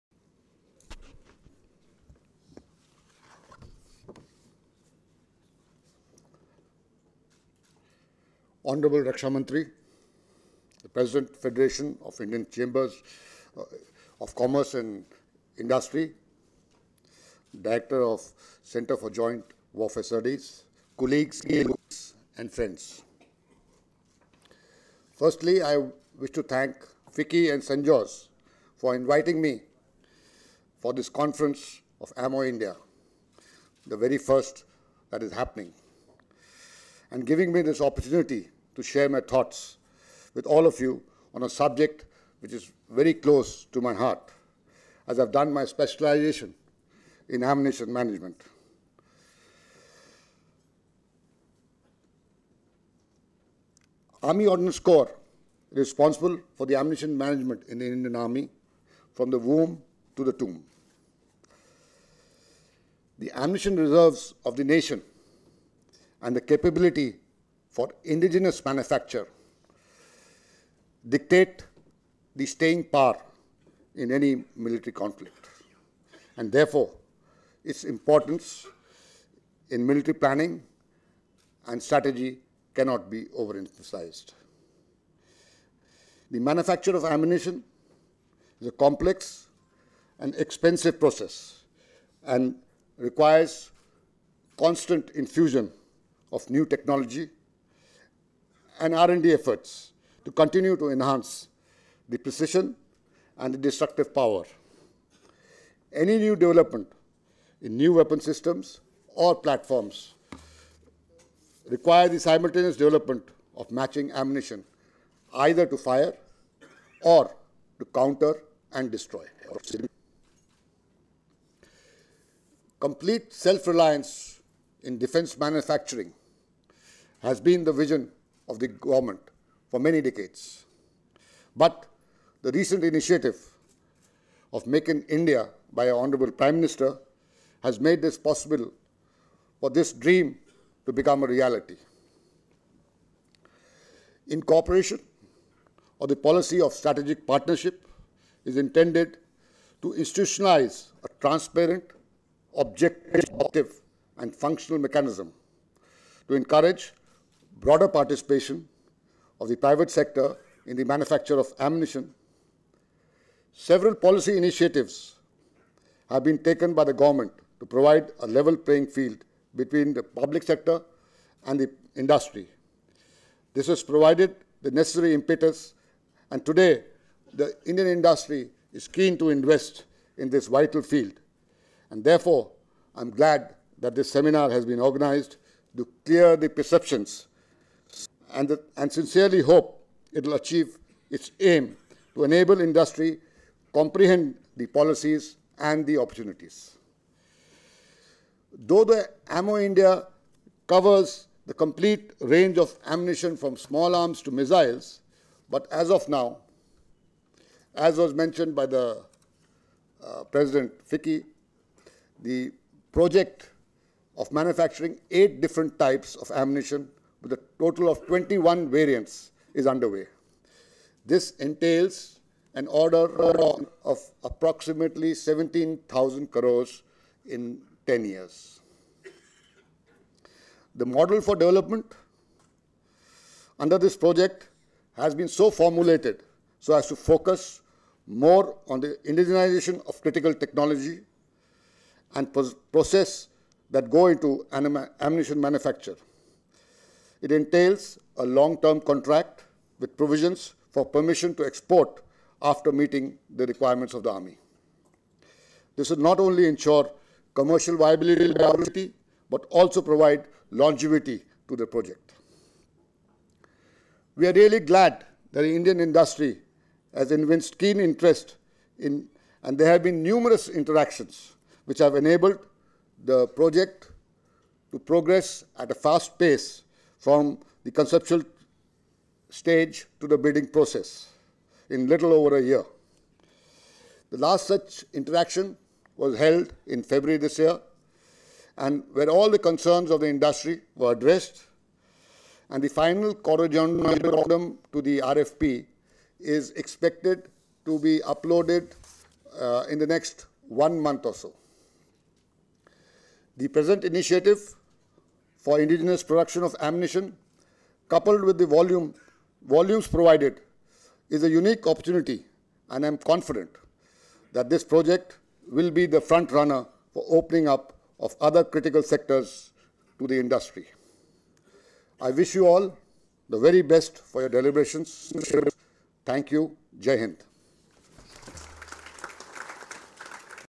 AmmoIndia2018 Special Address,Lt Gen Giri Raj Singh SM, Dir. General Ordnance Services,Indian Army
Lt Gen Giri Raj Singh SM, Director General Ordnance Services, Indian Army delivering the Special Address during AmmoIndia2018 conference at FICCI

credit FICCI INDIA